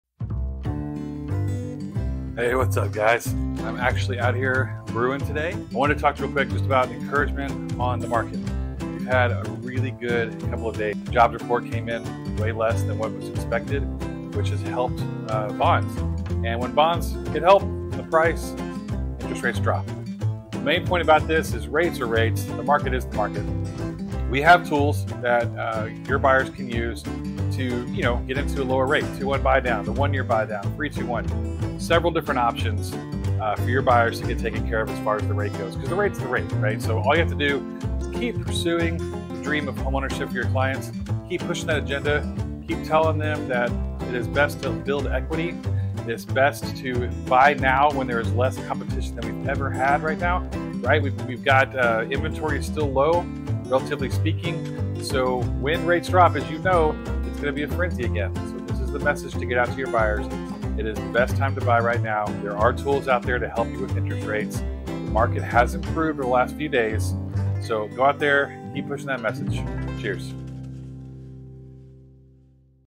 Maximizing Gains: Navigating Today's Interest Rates
There are several tools to win in today's higher rate environment.  The competition o buy a home is less than it's been in several years.  This means that sellers are willing to play ball and help buyers with concessions to cover closing costs or buying the interest rate down.  Lenders have several tools available to help buyers with temporary buy-down programs.  The cost of waiting is way more than the cost of a refinance down the road when rates drop.

Travis Scott
The Mortgage Brewer
powered by Summit Lending
NMLS 262604